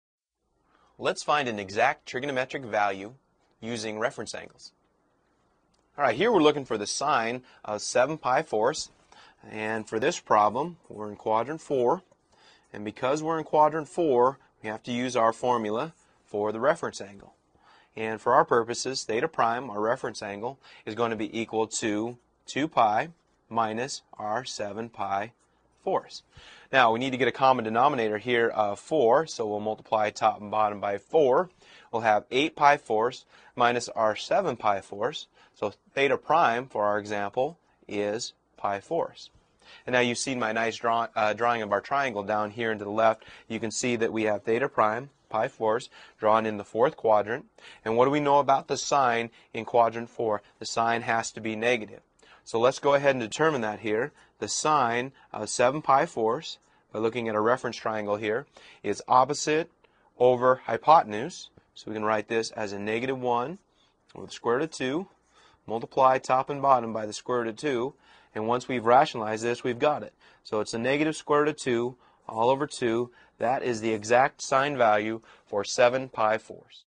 Chapter Test Prep – Ch 1, Exercise 9
An instructor walks you step-by-step through the solution to Exercise 9 in the Chapter 1 Test from the textbook Blitzer Trigonometry 7e.